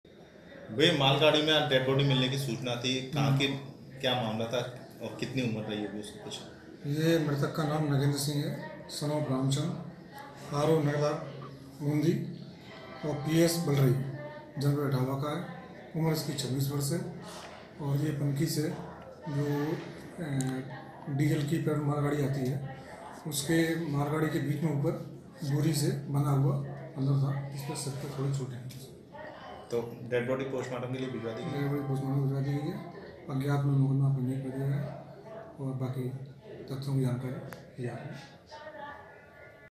मालगाड़ी में बंधा मिला युवक का शव, जांच में जुटी जीआरपी
मथुरा: यूपी के मुथरा में डीजल से भरी मालगाड़ी में युवक का शव मिलने के बाद हड़कंप मच गया. शव मिलने की सूचना के मौके पर पहुंची जीआरपी ने शव को कब्जे में लेकर पोस्टमार्टम के लिए भेजा. शव मालगाड़ी से बंधा हुआ था. फिलहाल जीआरपी ने अज्ञात लोगों के खिलाफ मामला दर्ज कर जांच शुरु कर दी है.